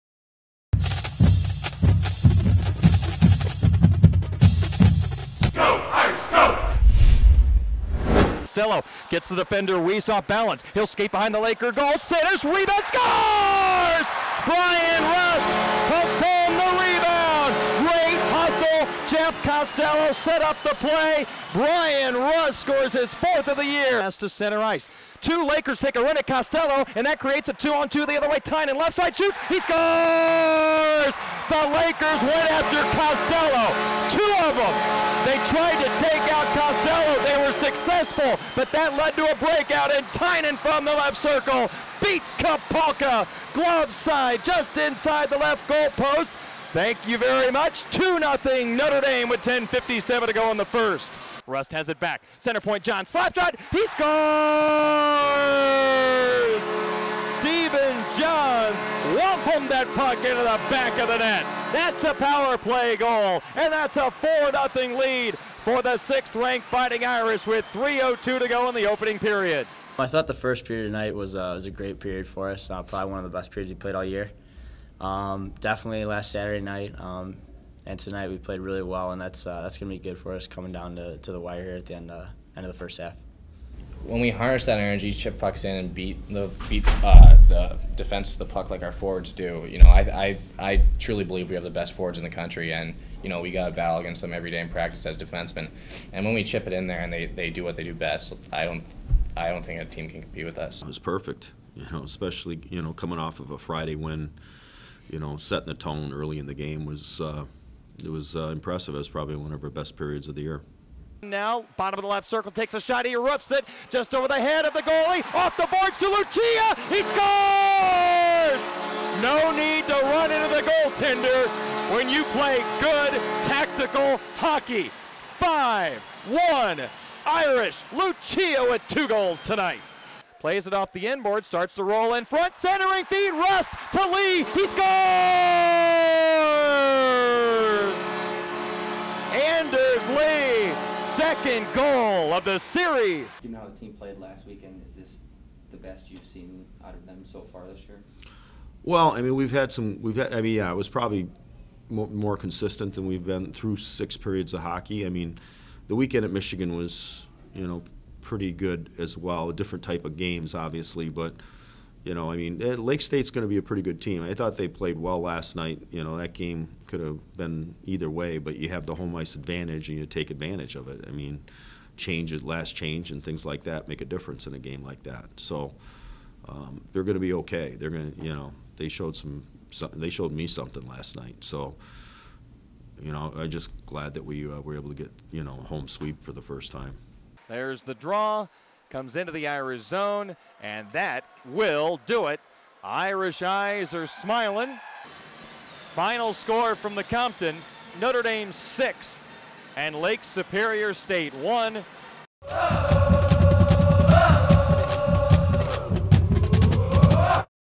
Earning the Home Sweep - Irish Defeat Lakers 6-1
The Notre Dame Hockey team earned their first home sweep of the season Saturday night after a 6-1 victory over the Lake Superior State Lakers at the Compton Family Ice Arena.